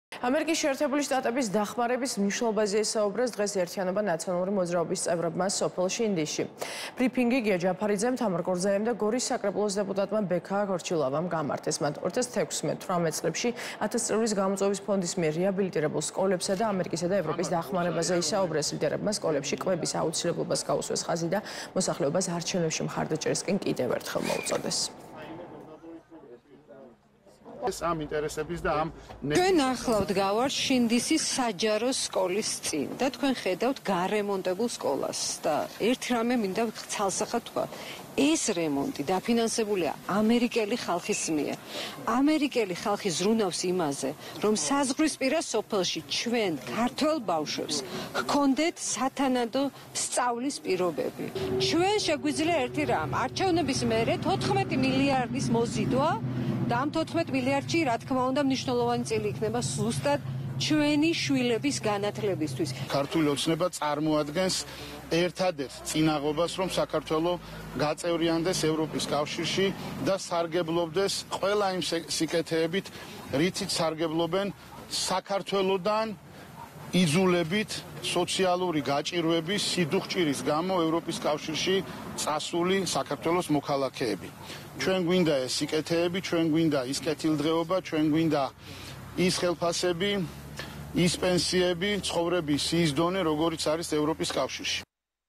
„ერთიანობა - ნაციონალური მოძრაობის“ განცხადება
ამერიკის შეერთებული შტატების დახმარების მნიშვნელობაზე ისაუბრეს დღეს „ერთიანობა - ნაციონალური მოძრაობის წევრებმა სოფელ შინდისში. ბრიფინგი გია ჯაფარიძემ, თამარ კორძაიამ და გორის საკრებულოს დეპუტატმა ბექა ქორჩილავამ გამართეს. მათ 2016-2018 წლებში „ათასწლეულის გამოწვევის“ ფონდის მიერ რეაბილიტირებულ სკოლებსა და ამერიკა-ევროპის დახმარებაზე ისაუბრეს. ლიდერებმა სკოლებში კვების აუცილებლობას გაუშვეს ხაზი და მოსახლეობას არჩევნებში მხარდაჭერისკენ კიდევ ერთხელ მოუწოდეს.